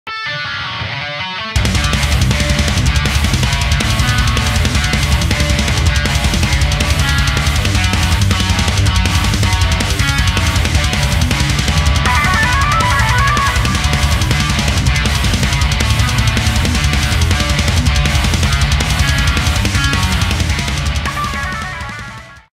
Eonia Preproduction
Sorry I haven't been uploading as much guys. In my spare time I've been working on some demos and I plan on recording vocals with some friends soon so here's a sneak peak of that.